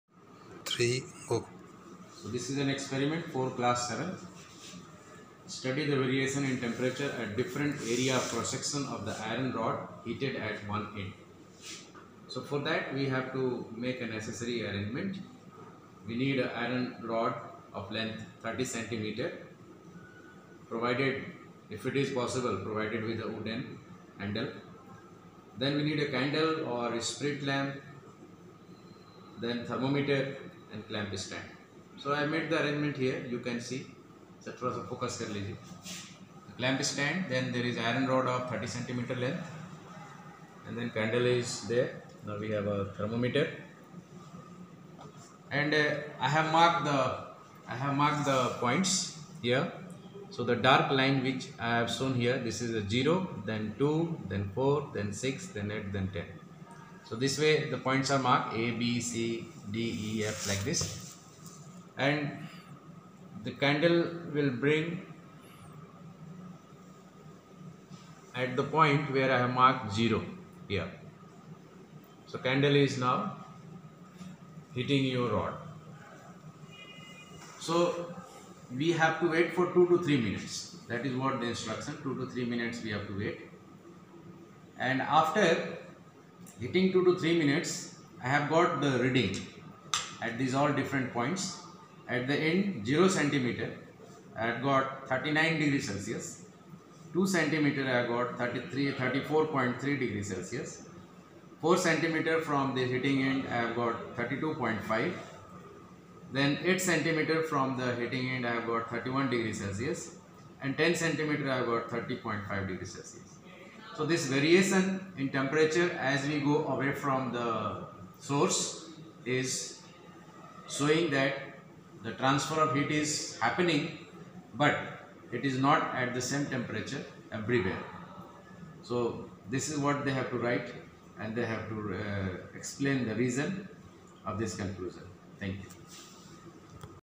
Conduction of Heat and variation of temperature along the length
This experiment shows the transfer of heat by conduction method and variation of temperature along the length of the rod. As we move away from the heating end,  temperature decreases.